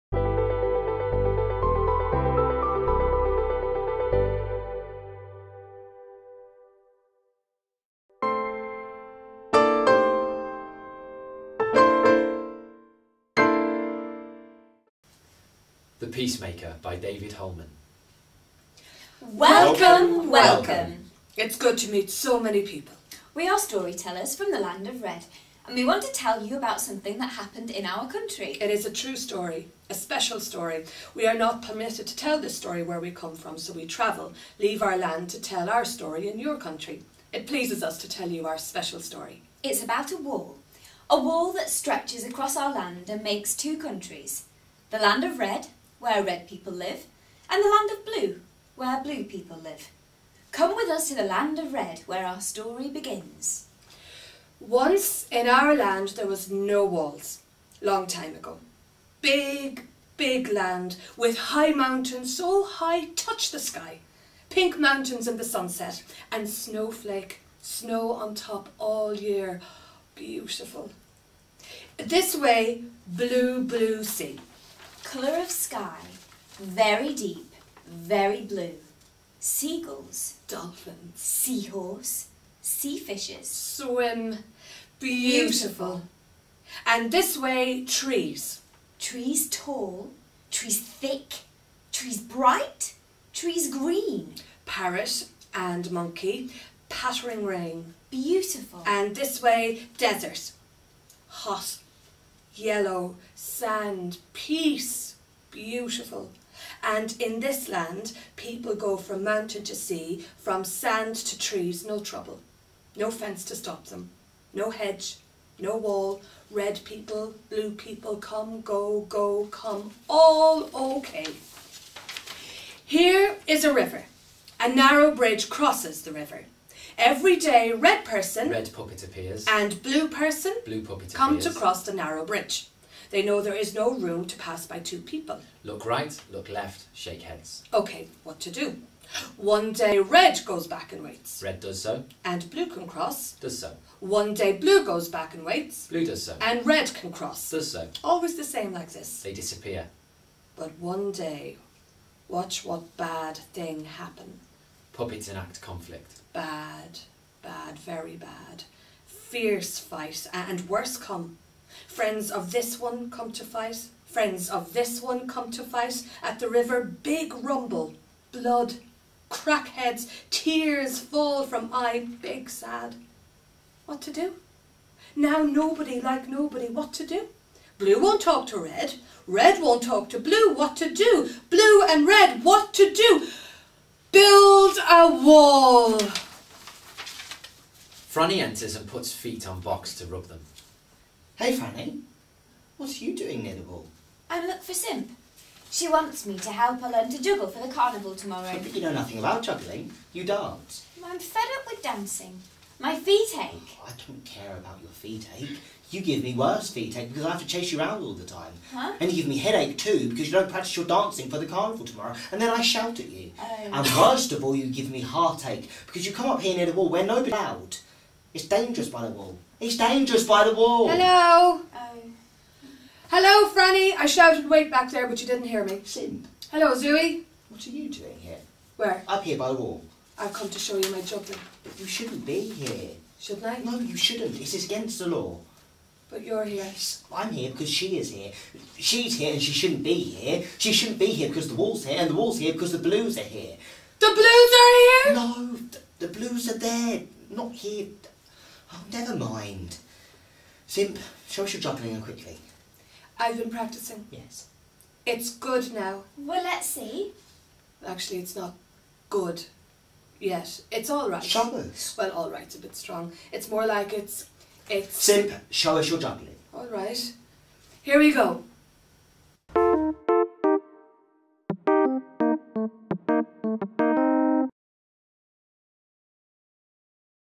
Skylines Extract Series: Peacemaker
A play extract in our Skylines series of resources for writers.

This video shows an extract of Peacemaker by David Holman, for ages 4+.

Skylines is Theatre Centre's online resource for playwrights who would like to know more about writing plays for children and young people.

Find out more and visit the full set of resources